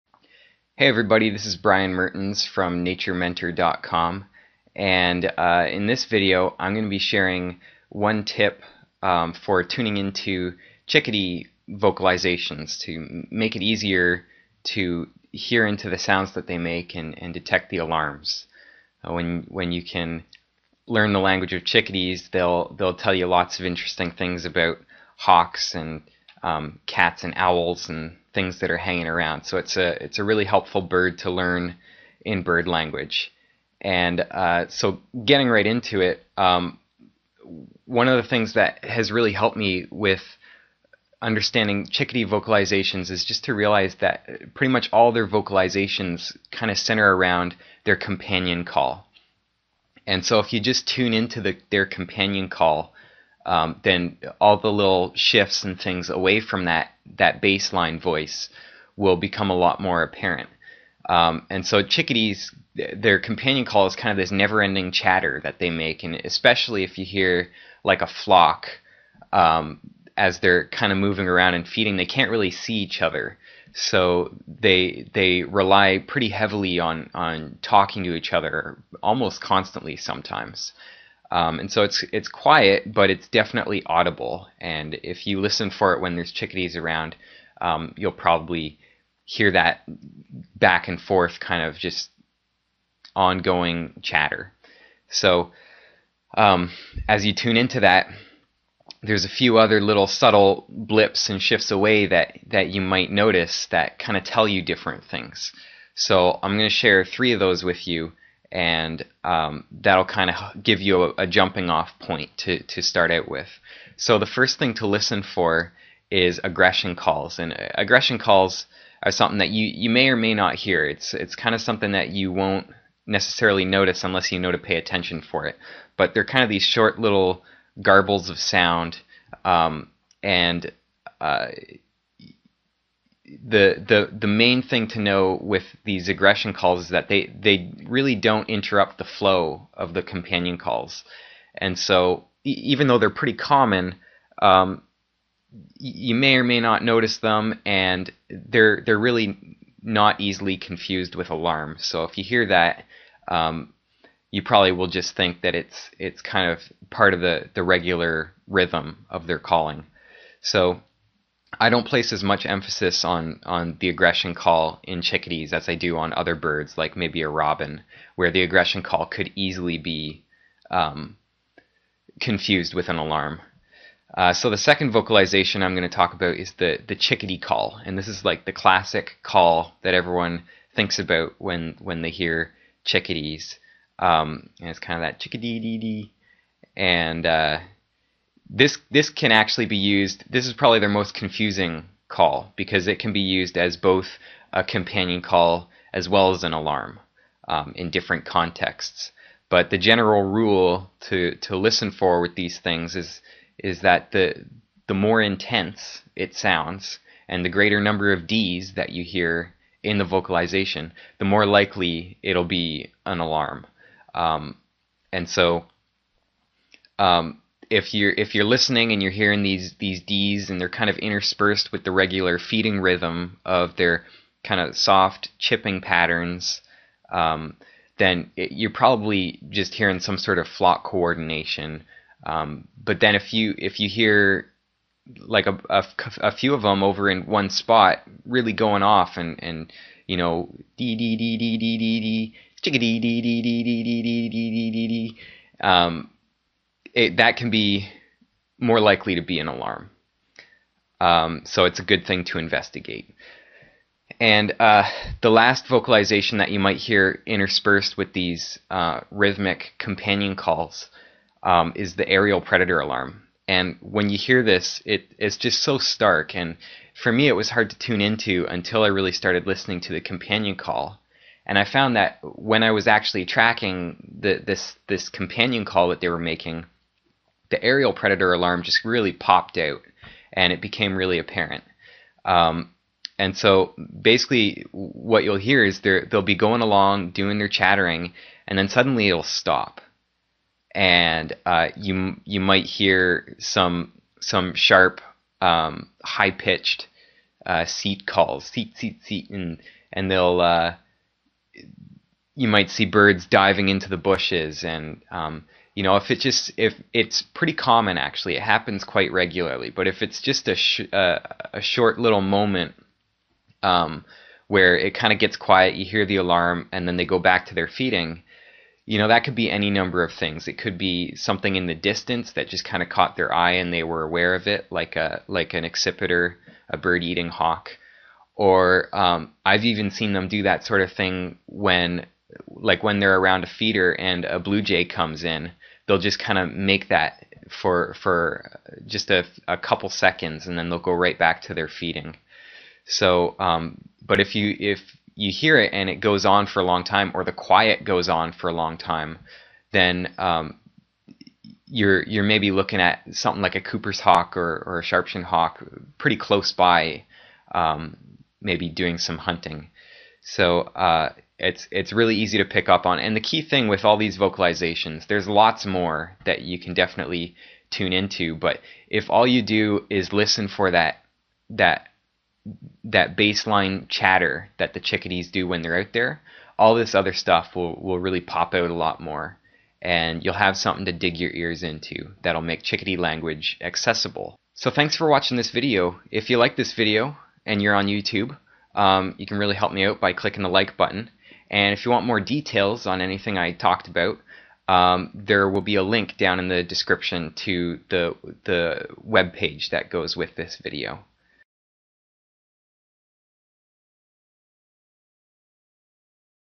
Tuning Into Chickadee Calls
In this video I share a simple way of tuning into chickadee calls that will help bring out an awareness of the alarm calls that chickadees broadcast.  You can use these vocalizations to track down predator animals like hawks, owls, and cats.